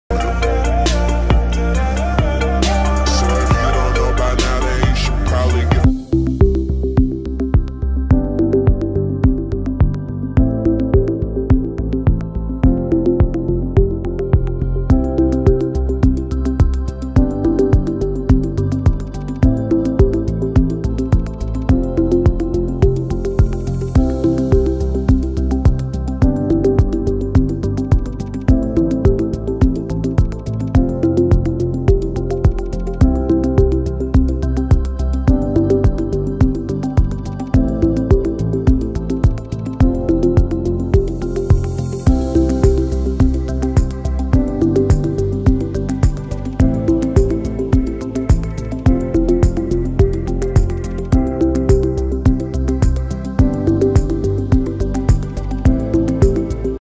18 inch cutter suction dredger LD3700 cutter head test
Different people and different cutter suction dredgers have their unique characteristics. We are proud of these dredgers that we produce.
The video shows the cutter head test of the LD3700 cutter suction dredger in the factory.
leader dredger